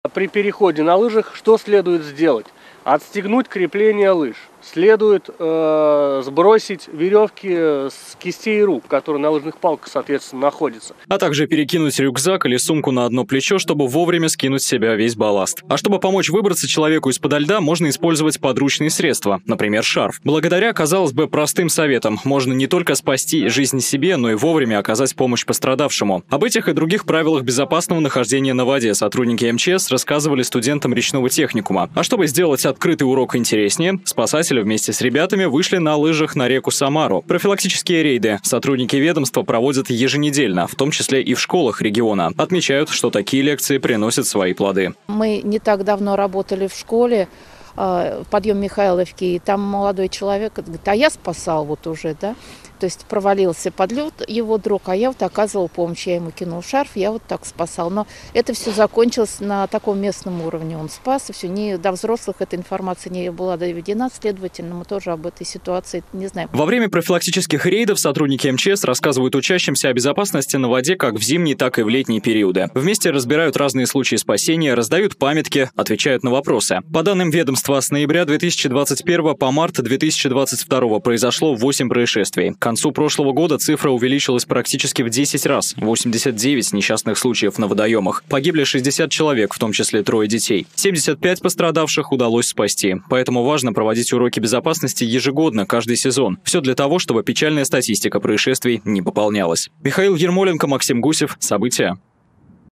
Сотрудники МЧС провели для студентов речного техникума открытый урок
Перешли реку на лыжах и узнали правила безопасного нахождения на водоеме. Сотрудники МЧС провели для студентов речного техникума открытый урок, во время которого учащиеся получили инструкцию о правильном переходе рек и озер в зимнее время, узнали как оказывать первую помощь и как вести себя, если провалился под лед. Вместе со студентами на лекции побывал Михаил Ермоленко.